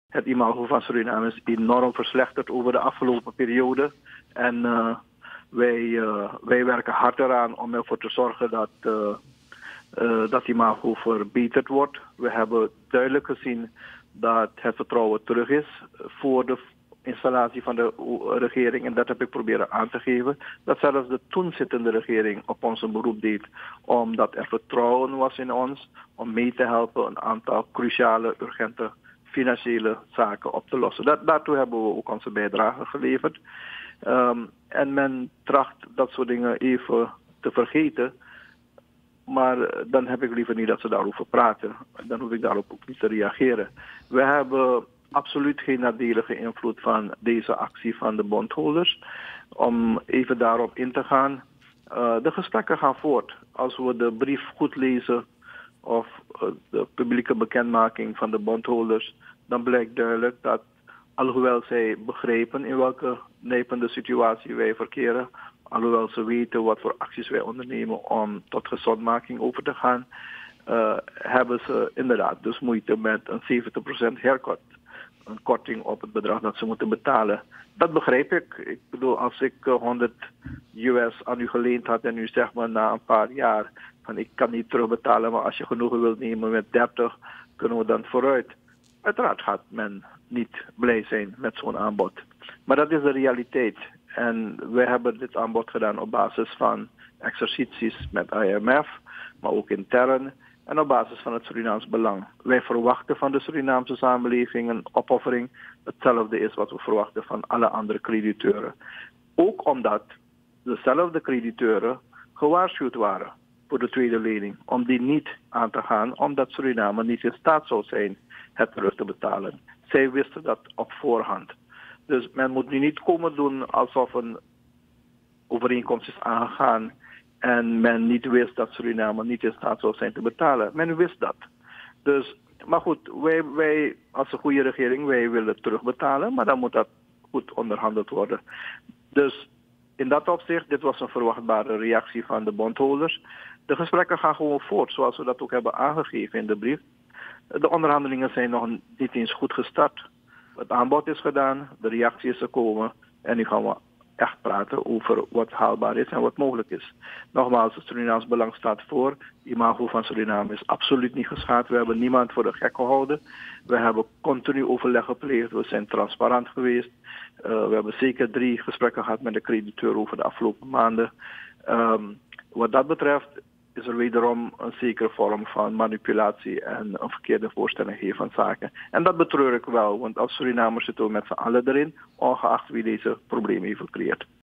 Gesprekken met Oppenheimer-bondholders worden voortgezet - ABC Online Nieuws
ABC Online Nieuws - 16 juni 2021

Gesprekken met Oppenheimer-bondholders worden voortgezet

Minister Albert Ramdin was er niet over te spreken gisteren dat NDP parlementariërs de regering beschuldigden het imago van Suriname internationaal te hebben geschonden bij de onderhandelingen met de crediteuren.

Volgens Ramdin werkt de regering juist erg hard aan de verbetering van het imago van ons land. Wat de gesprekken met de Opperheimer-bondholders betreft, die gaan gewoon door.

“Wij gaan nu praten over wat haalbaar en mogelijk”, zegt Ramdin.